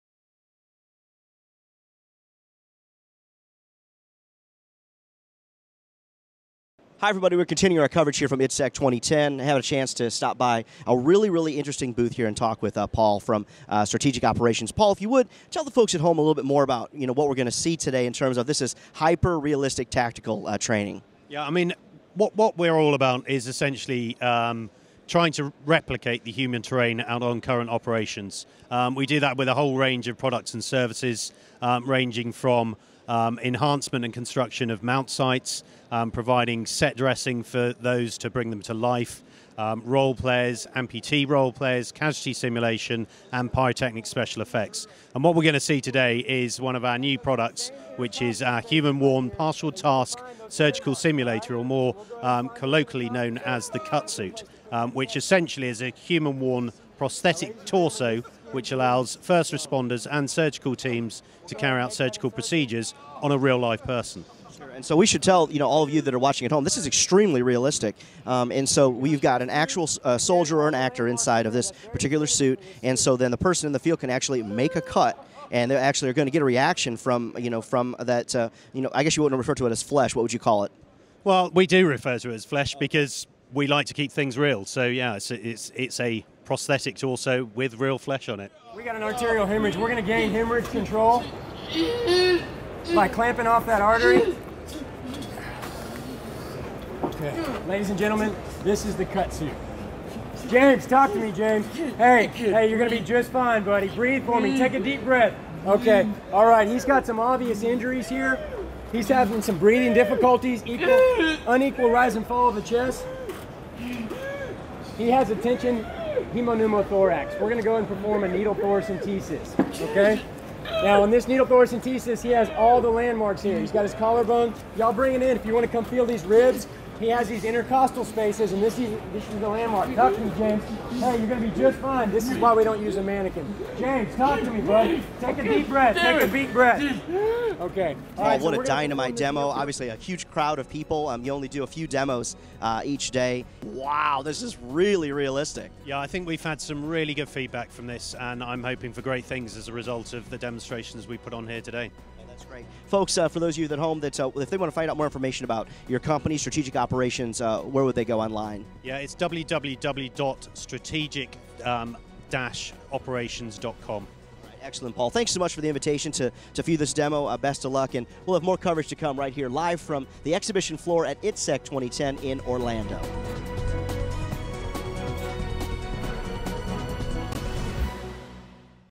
I/ITSEC 2010, The Cut Suit by Strategic Operations
Hyper realistic military medical simulation at I/ITSEC 2010 in Orlando.  Real person wears a suit of flesh so that medics can practice cutting artificial flesh in a real life simulation.